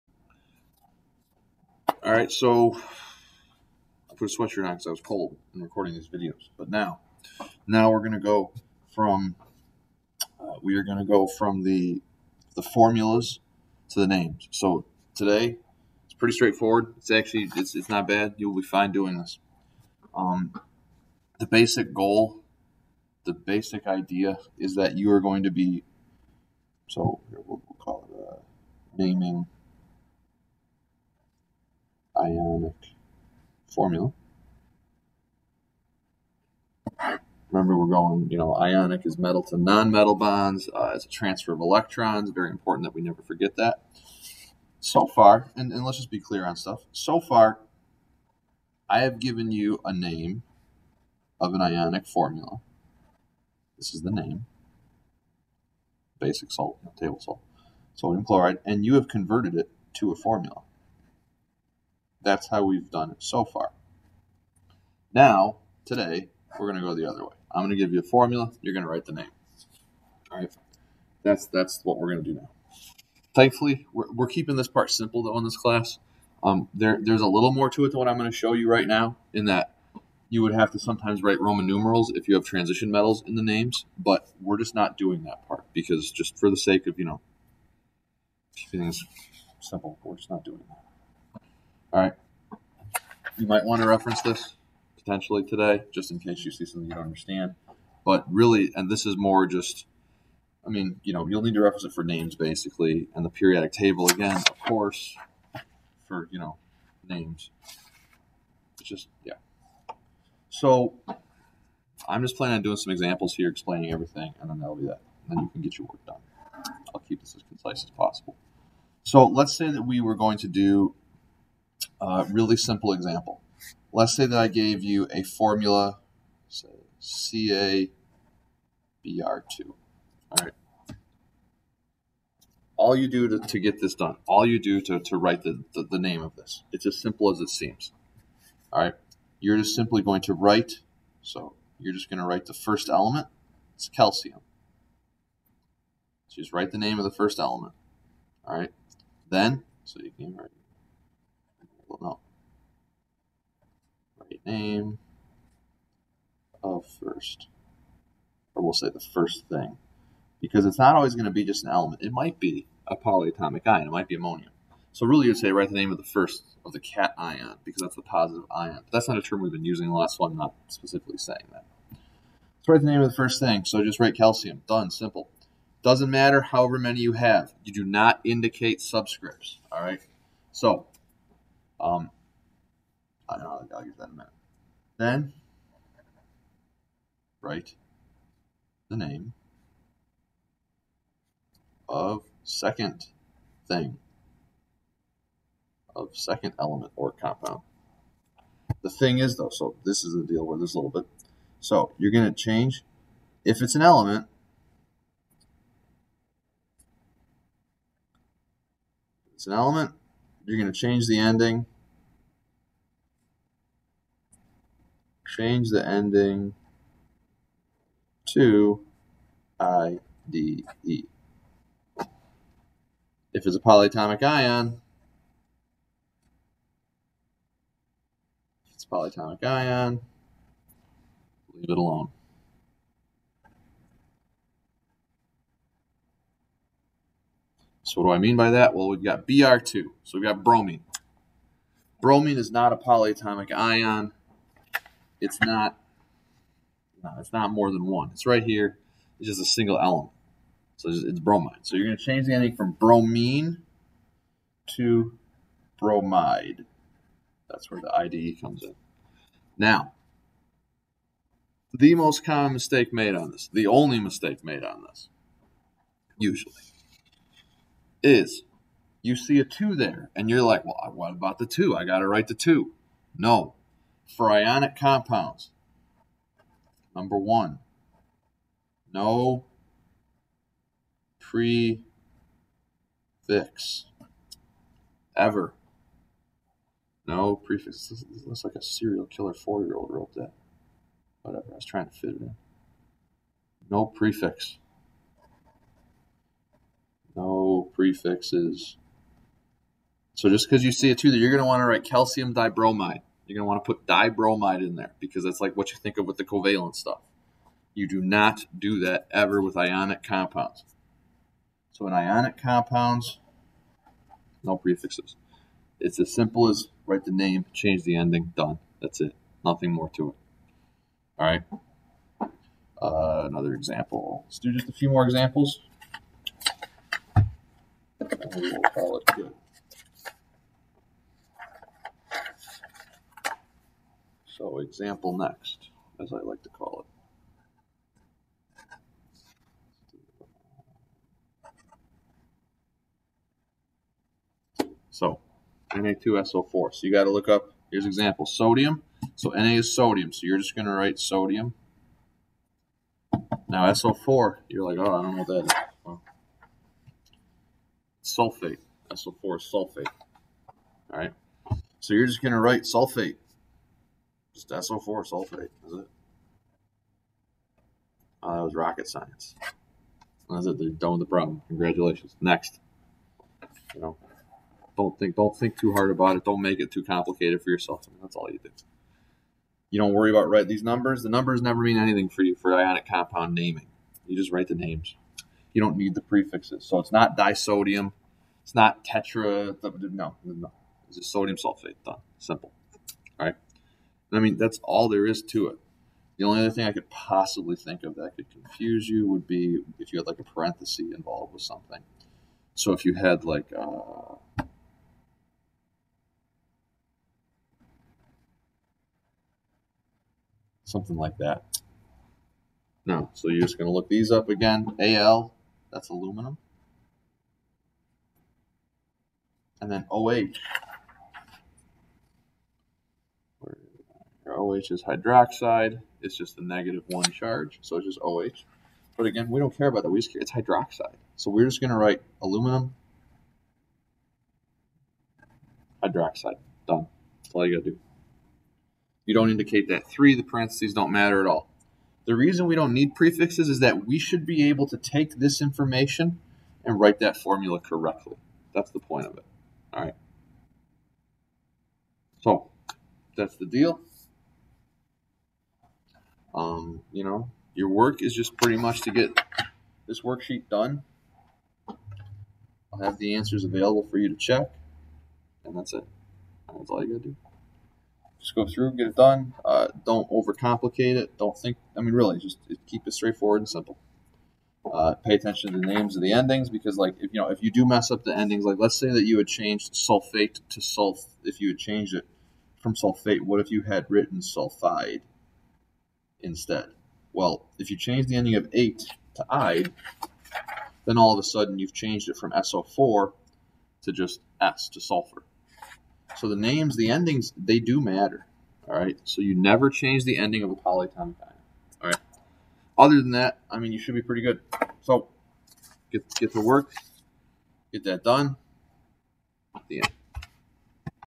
Naming Ionic Formula
How to write the names of binary ionic formula.